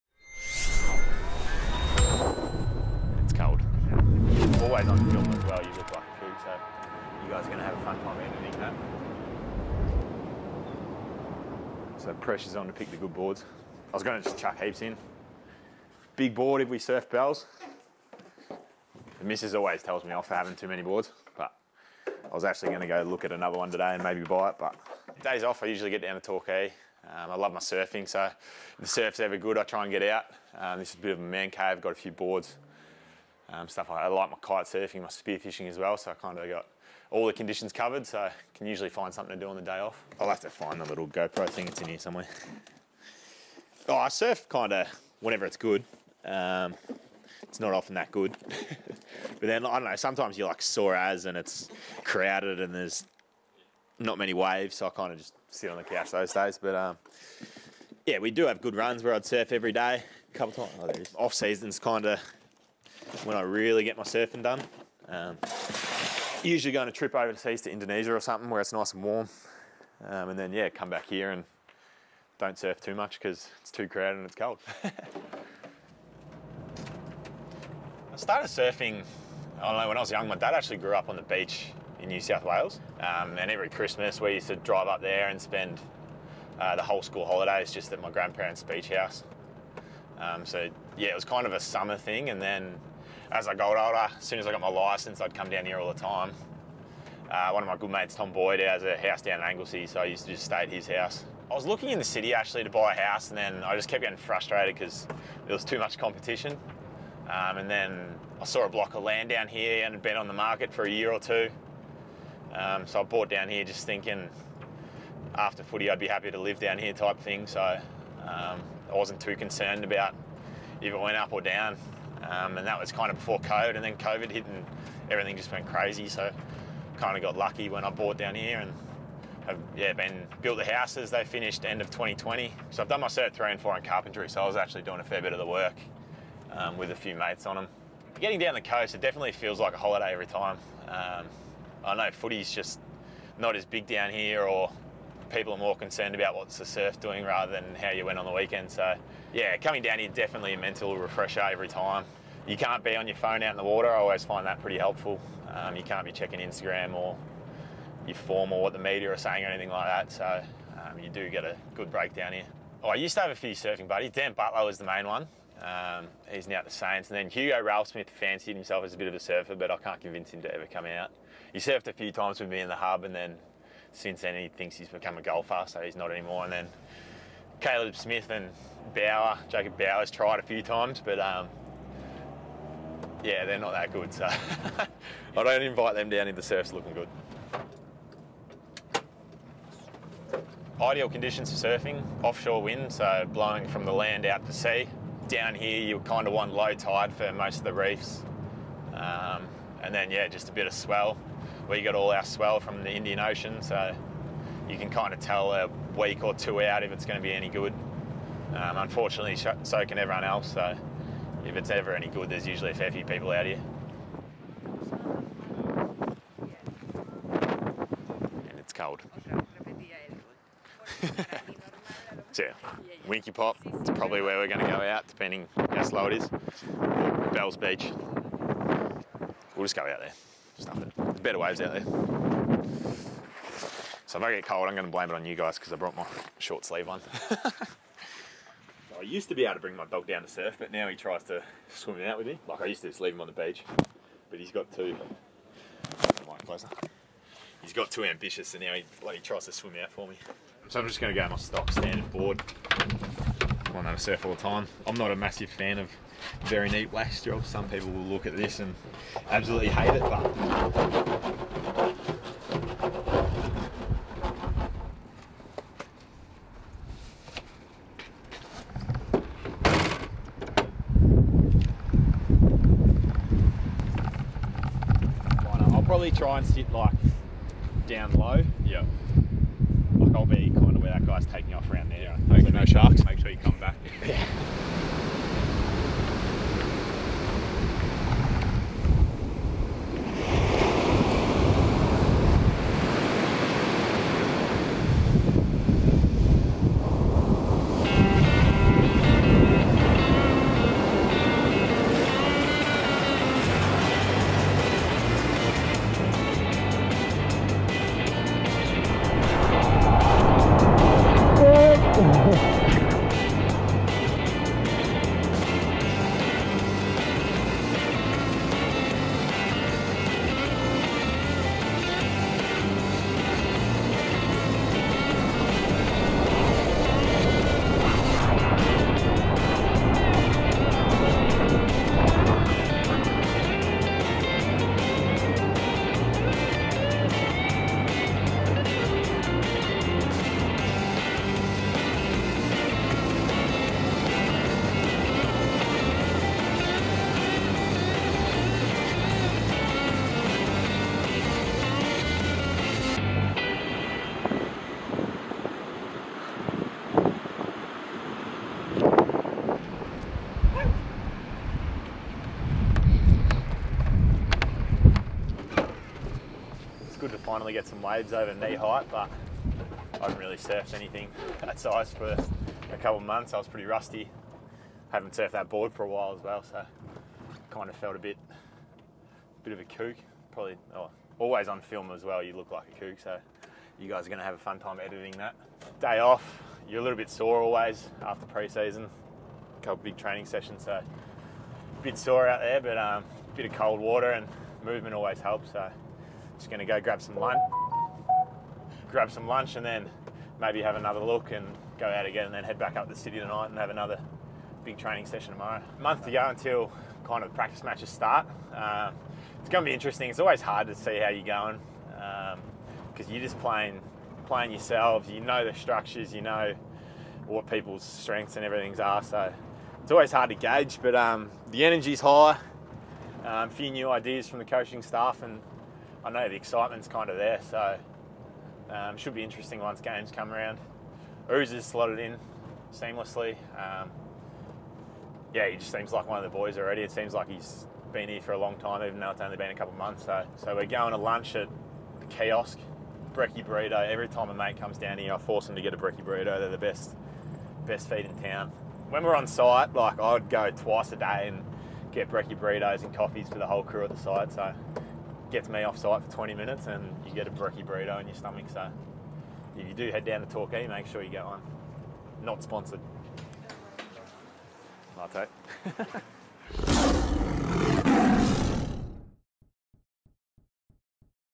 ROAR: Riding the waves
Richmond Defender Nick Vlastuin has a love for surfing, a way to take his mind off football in his second home, Torquay.